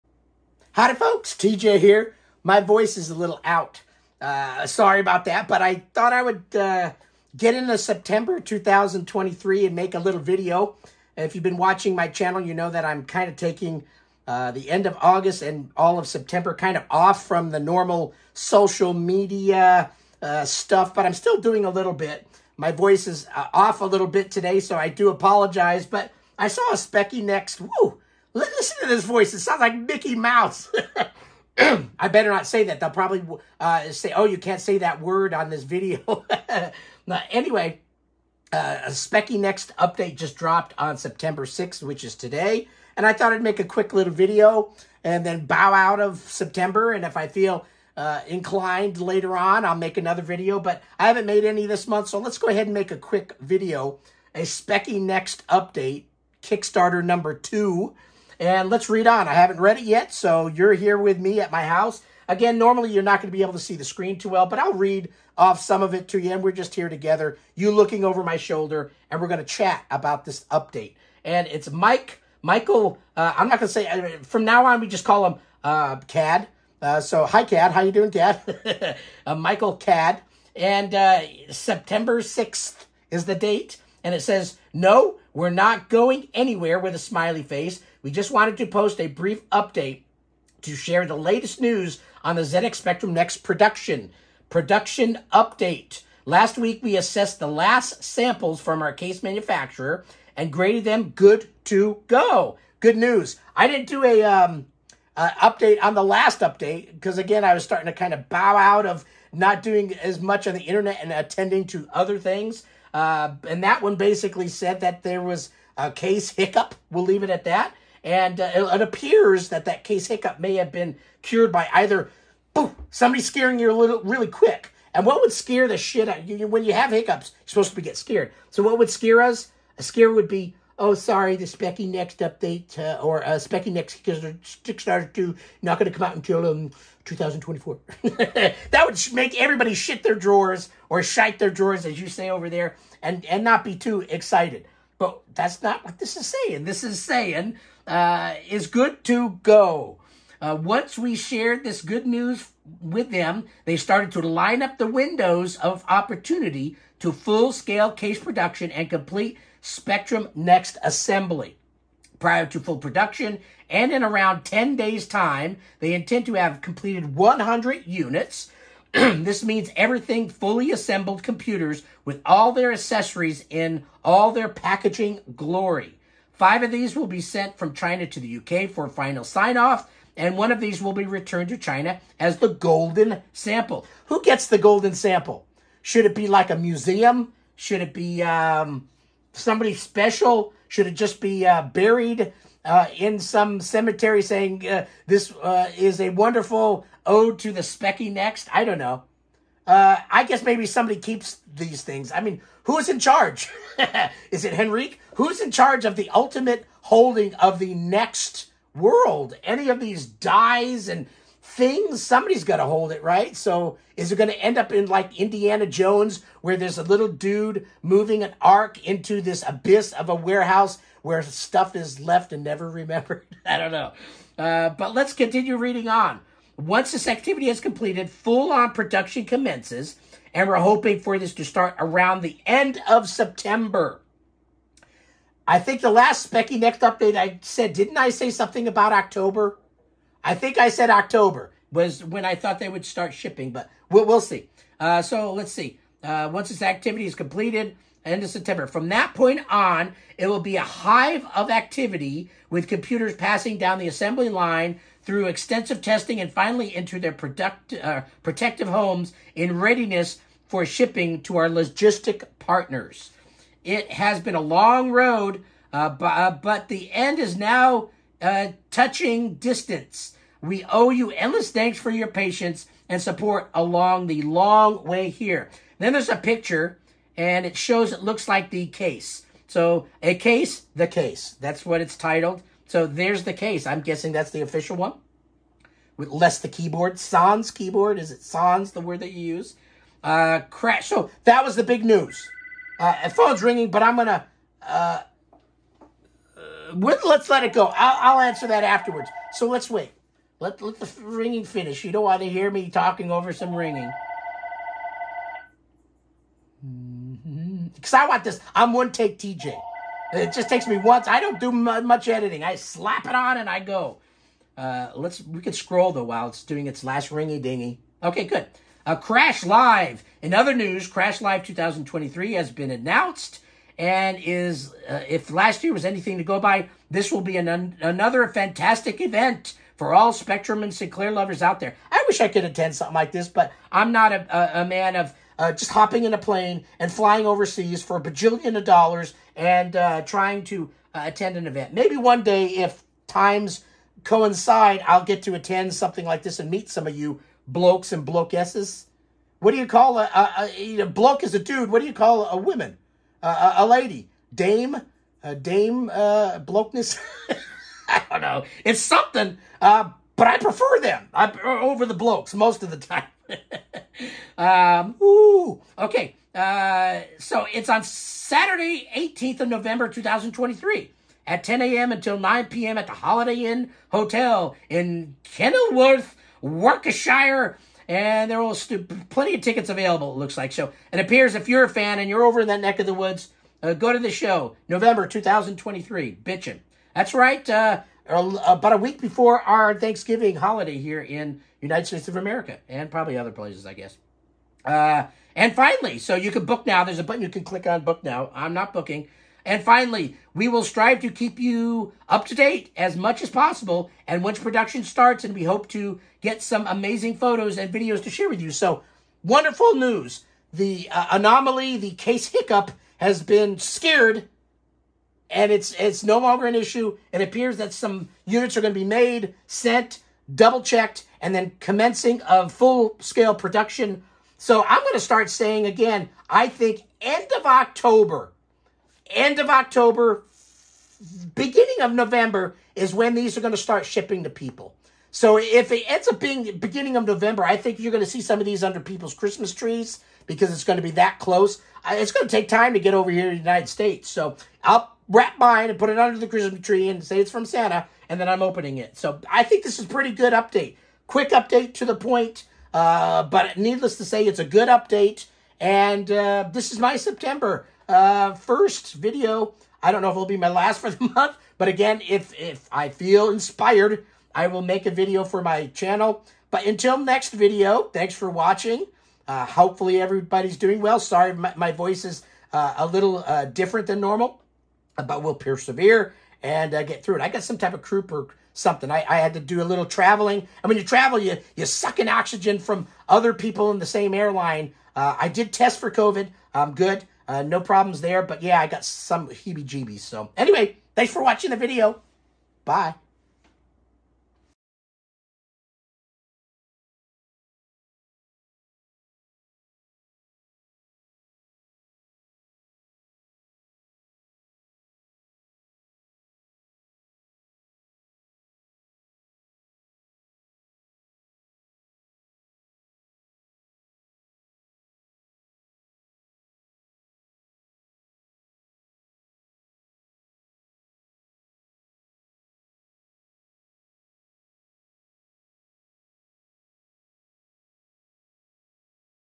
Sinclair ZX Spectrum Next Kickstarter #2 Update #32 - September 2023 - All Engines Go ?
The Speccy Next Kickstarter #2 Update #32 has dropped and figured now was a nice time to pop in and do a quick video.

Nice to see you all again.  Chime in on the messages below and let us chat about this new update from the Next team.

Do you enjoy my YouTube Channel?
Do you want even more fun 8Bit coverage?